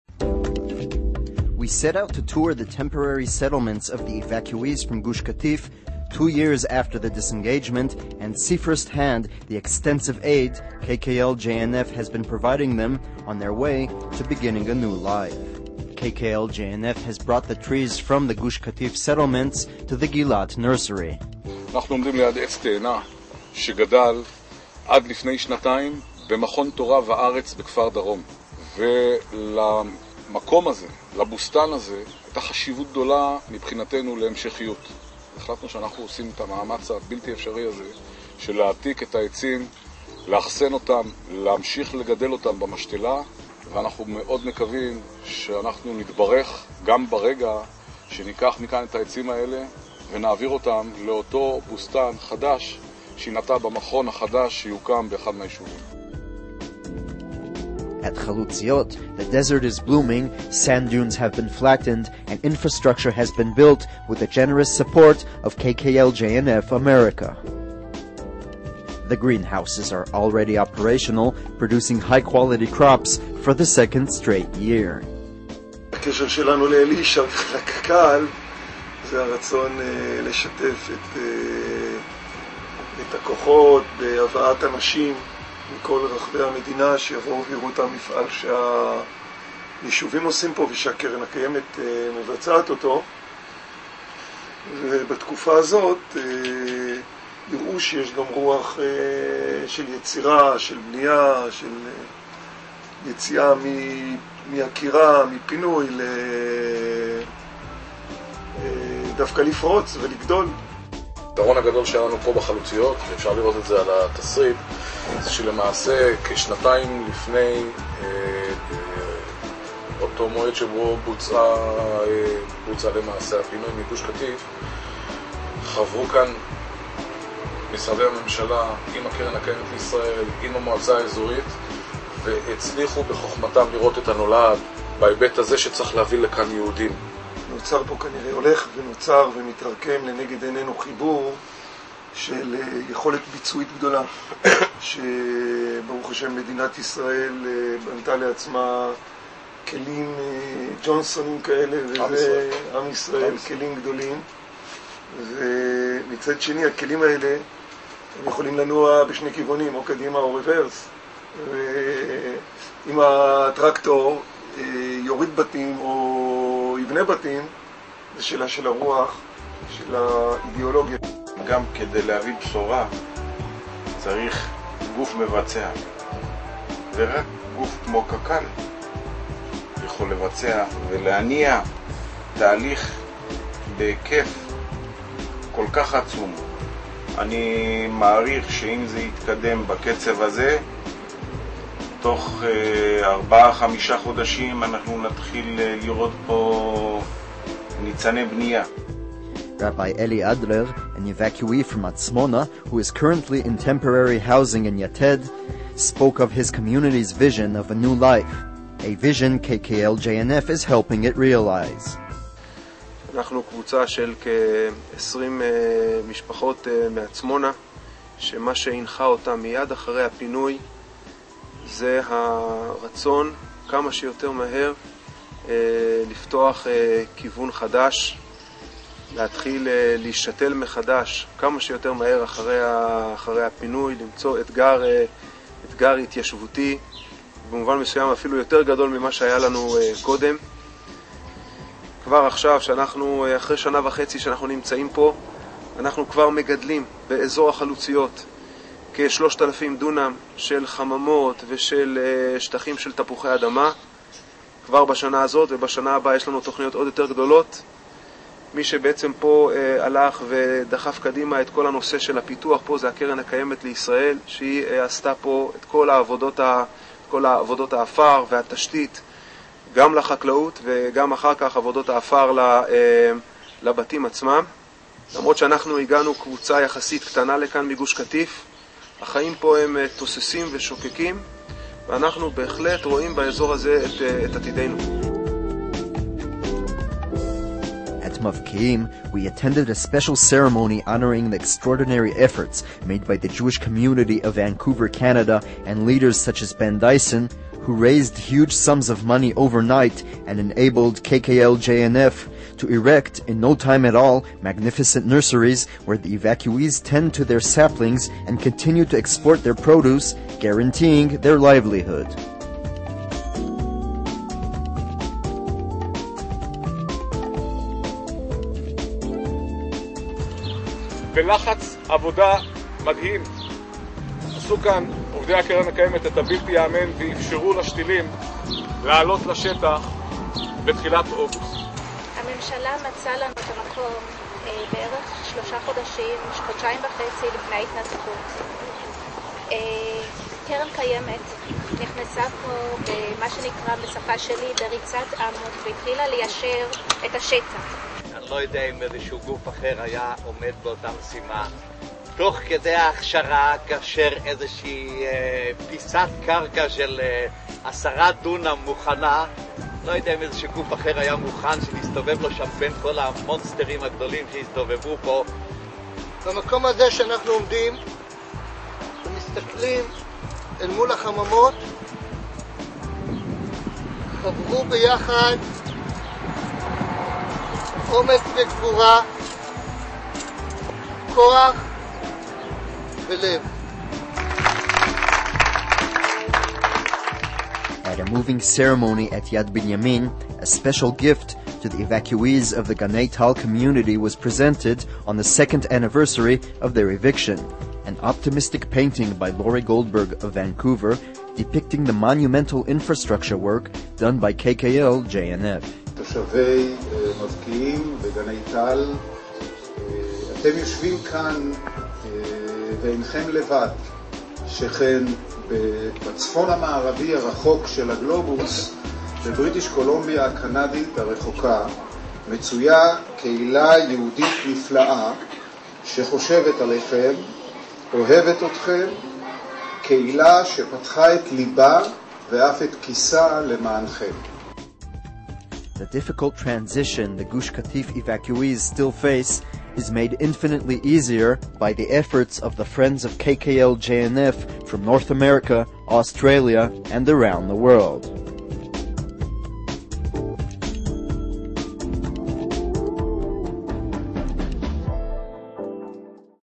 KKL-JNF builds the dream of the Gush Katif evacuees
Two years after the Disengagement, KKL-JNF, with the generous support of its friends worlwide, is helping the Gush Katif evacuees realize their dreams in the western Negev by building greenhouses and preparing the ground for the day they will enter their permanent residences.